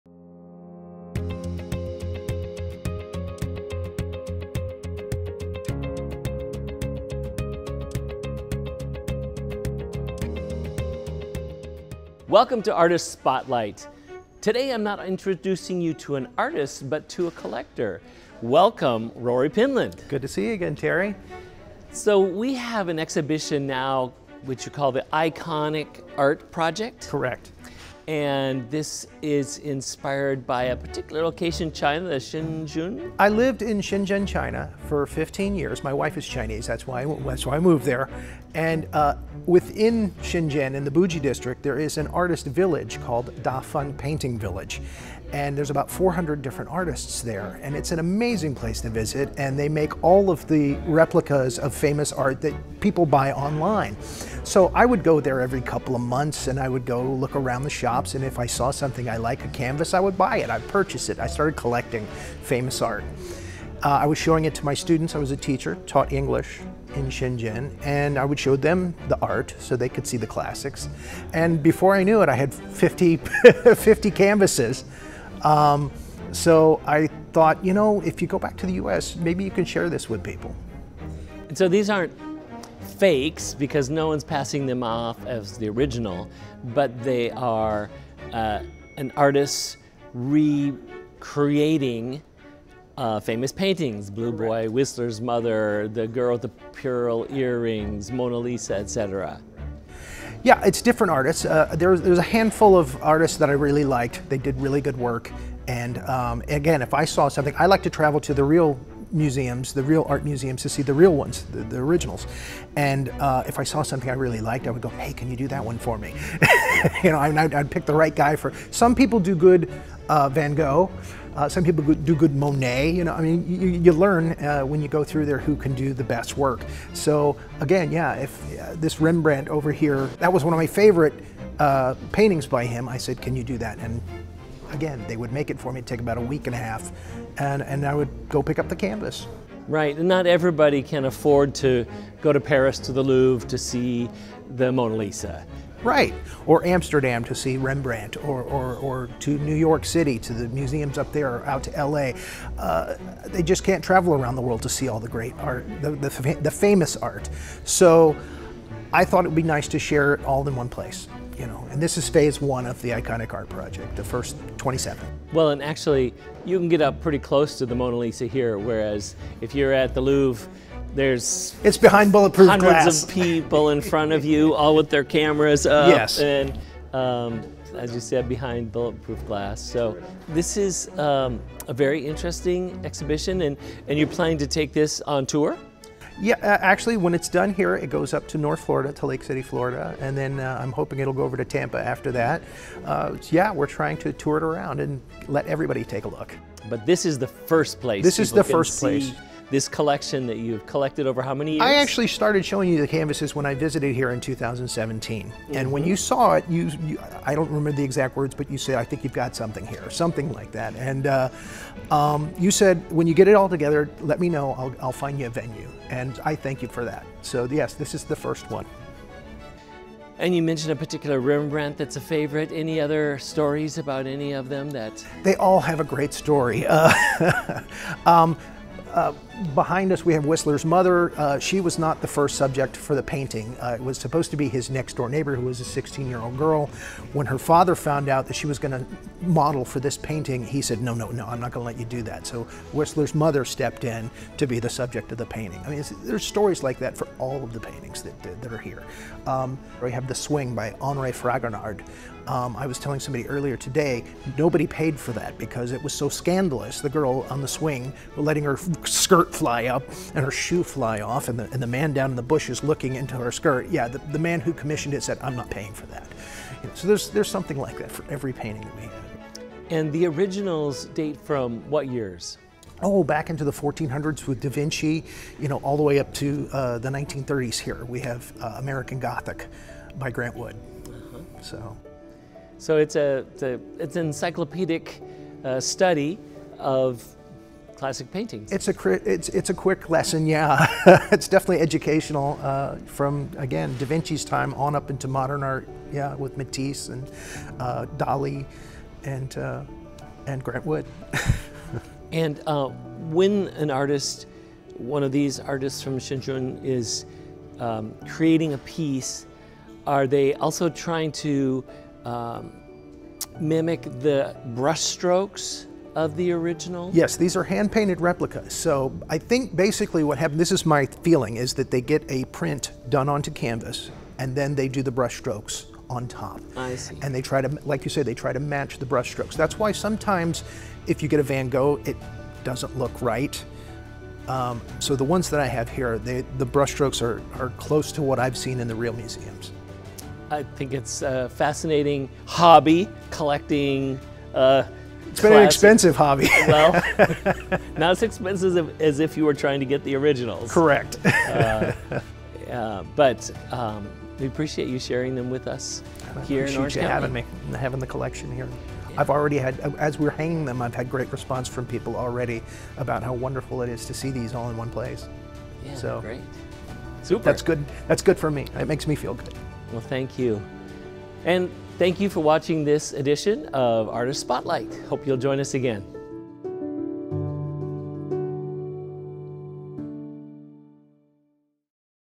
Artist Spotlight | Rory Penland
Orange County Arts and Cultural Affairs unveiled a stunning exhibition of quality iconic art replicas collected and curated by Rory Penland. The Iconic Art Project was born when Penland began visiting DaFen Painting Village in the Buji district of Shenzhen China in 2014. The Village has over 400 art shops creating finest quality replicas of the world's finest art.

Prepare to be amazed as we showcase a collection of iconic art replicas from renowned artists around the world. Whether you're an art enthusiast, a casual admirer, or simply looking for a unique experience, The Iconic Art Project has something for everyone.